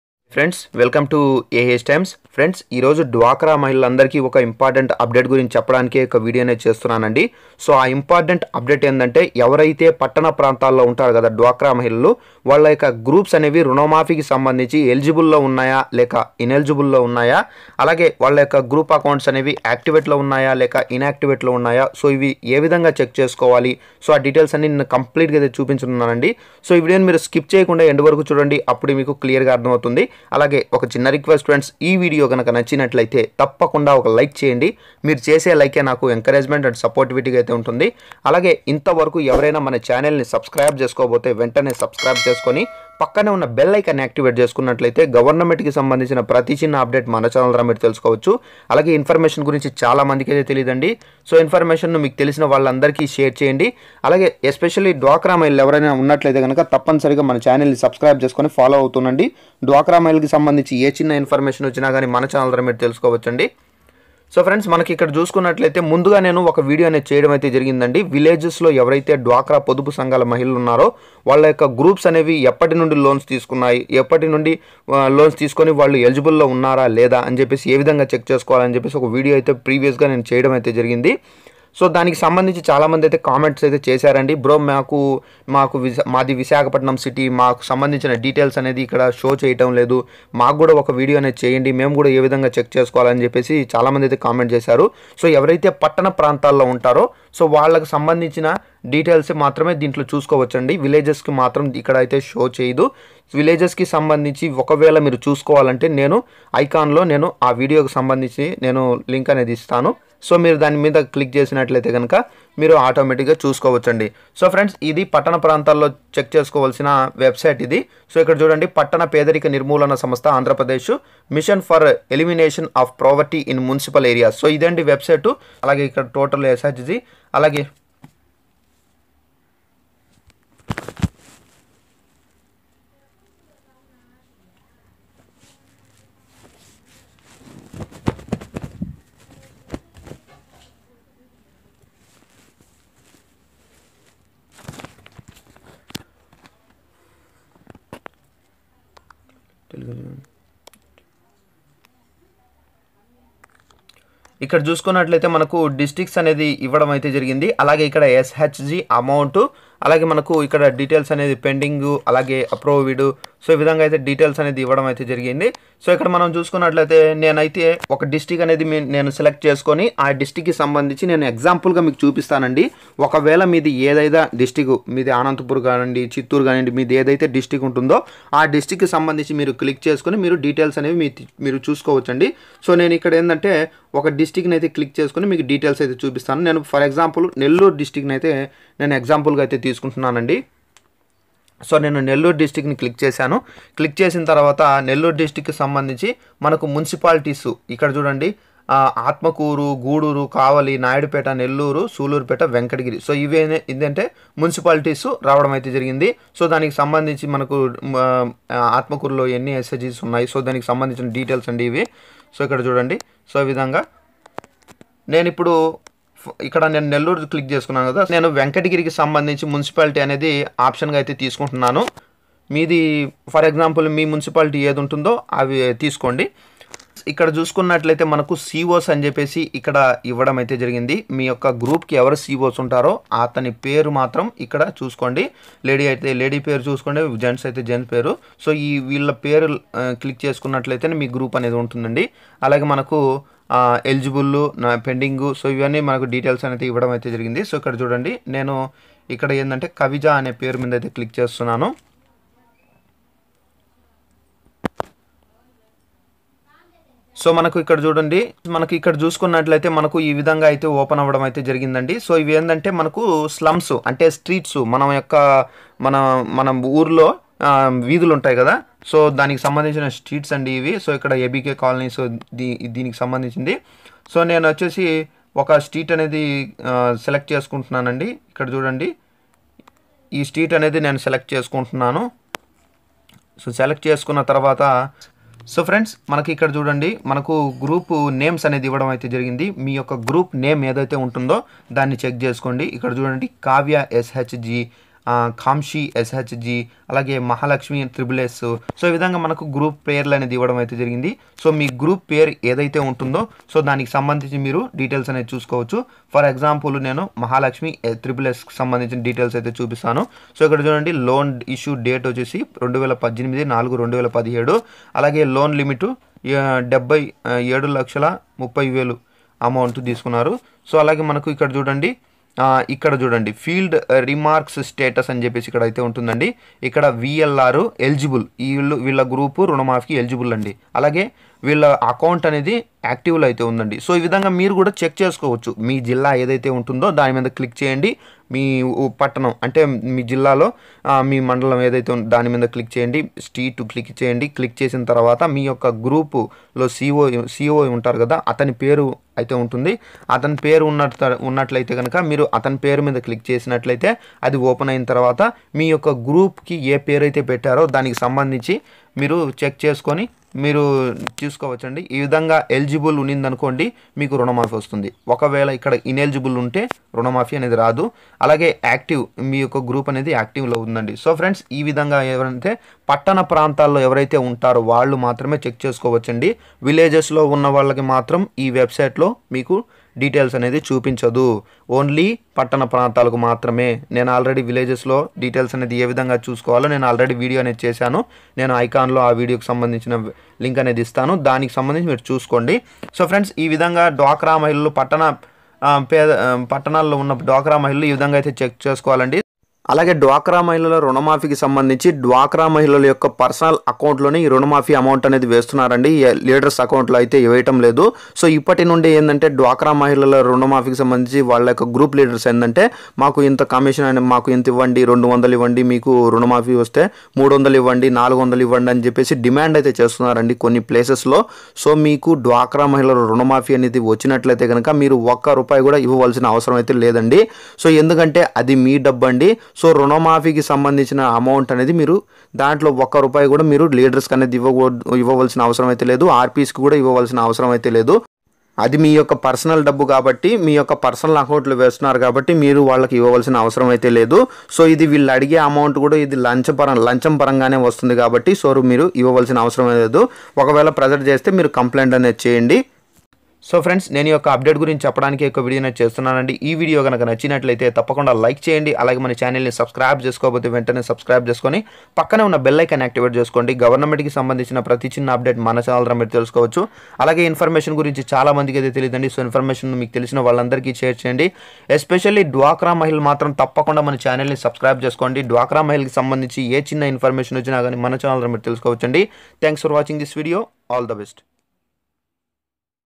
AP డ్వాక్రా రుణమాఫి అర్హుల లిస్ట్ మీ ఫోన్ లో ఇలా చెక్ చేసుకోండి పూర్తి వివరాలు తెలుసుకోండి
AP Dwakra Mahilalu Updates 2020